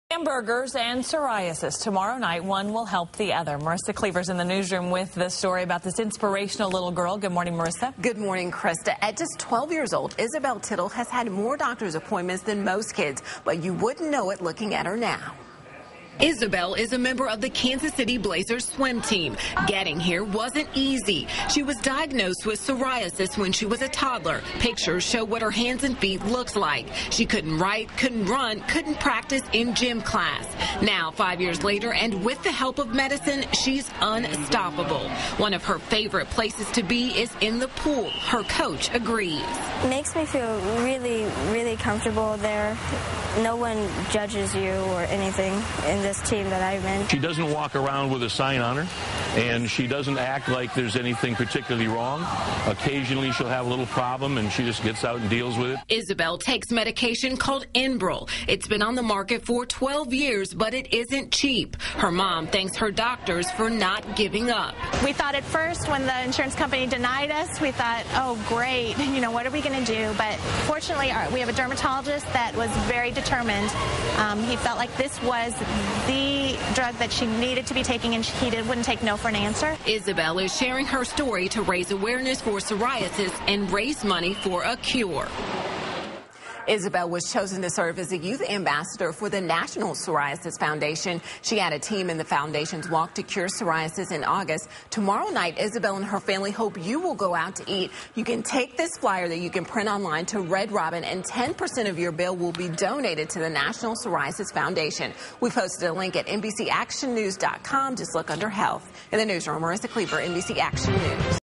How your hamburger can help psoriasis
Hamburgers and psoriasis. A Blue Springs girl hopes one will help the other.